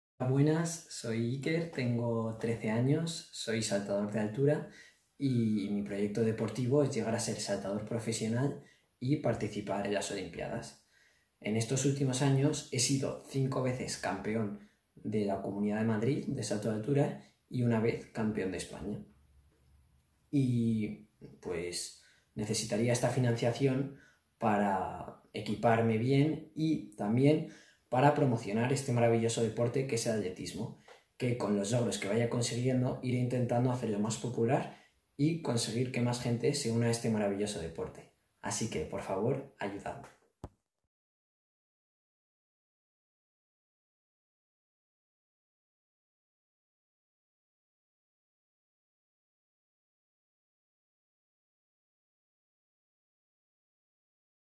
Iker Díaz Montilla / Promoción del atletismo y olimpiadas de salto de altura
Soy Iker, saltador de altura, y mi proyecto consiste en llagar a las olimpiadas de salto de altura y promocionar el atletismo para que cada vez más gente se una a este deporte. ¡Muchas gracias por estas becas!